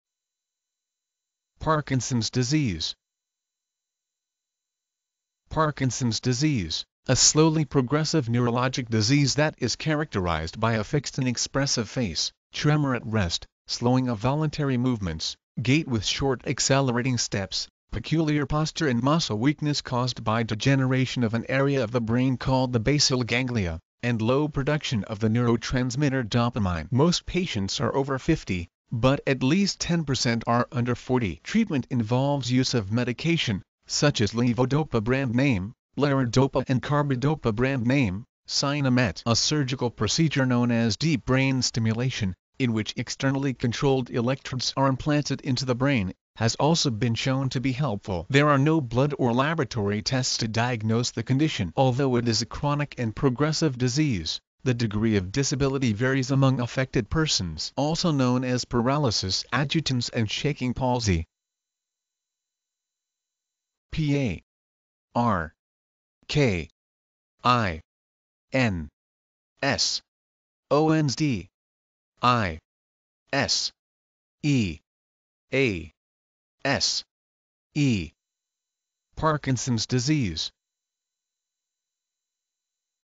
Parkinson's disease - Medical Definition and Pronunciation
Parkinson's disease

Parkinson's disease: A slowly progressive neurologic disease that is characterized by a fixed inexpressive face, tremor at rest, slowing of voluntary movements, gait with short accelerating steps, peculiar posture and muscle weakness (caused by degeneration of an area of the brain called the basal ganglia), and low production of the neurotransmitter dopamine. Most patients are over 50, but at least 10 percent are under 40. Treatment involves use of medication, such as levodopa (brand name: Larodopa) and carbidopa (brand name: Sinemet). A surgical procedure known as deep brain stimulation, in which externally controlled electrodes are implanted into the brain, has also been shown to be helpful. There are no blood or laboratory tests to diagnose the condition. Although it is a chronic and progressive disease, the degree of disability varies among affected persons. Also known as paralysis agitans and shaking palsy.